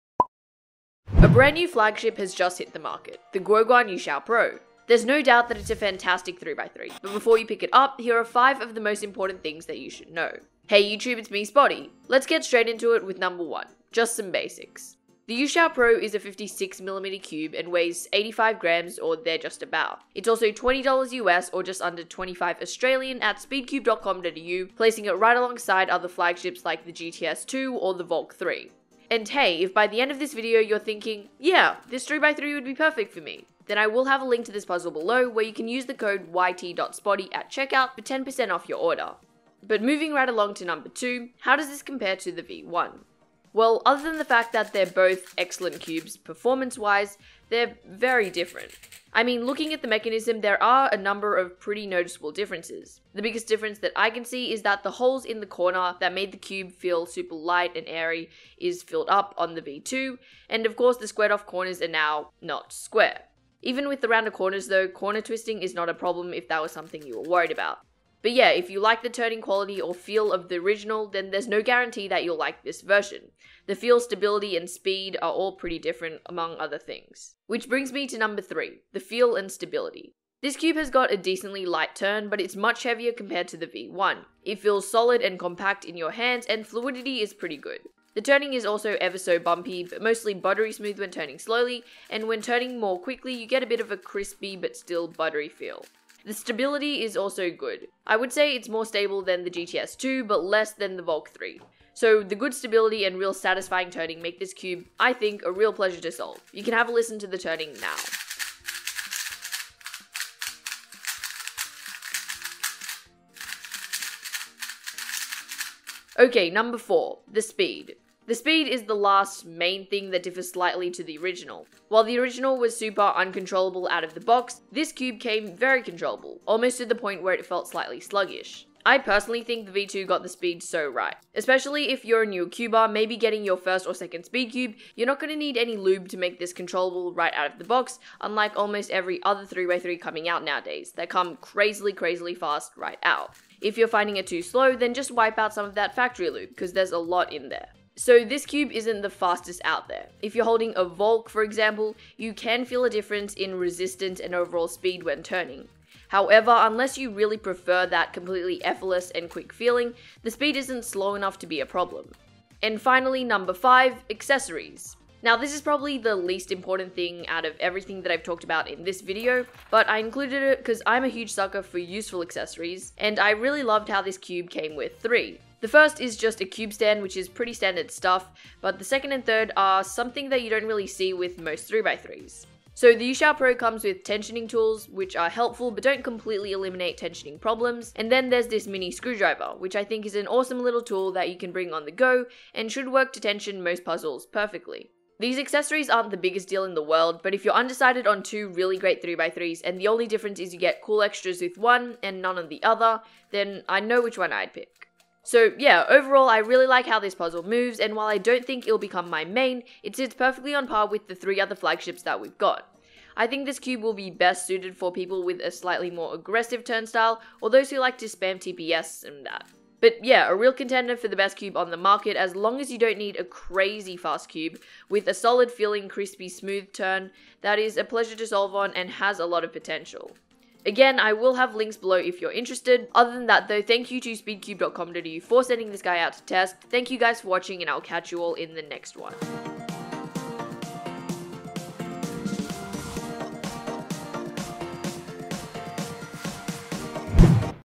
5 Things to Know: YueXiao Pro | Speedcube.com.au
5 Things to Know: YueXiao Pro | Speedcube.com.au

Hello! Today we've got the top 5 things that I think you should know before picking up the GuoGuan YueXiao Pro 3x3! I would really appreciate any constructive criticism on this video :D Also, thank you to speedcube.com.au for sending this puzzle out to test!

Stuff I use:
Camera: Olympus OMD EM Mark II
Lens: Olympus M.Zuiko 14-150mm f4-5.6 Mark II
Microphone: Samson Go Mic
Editing: Adobe Premiere Pro + Adobe Photoshop

- Spotty.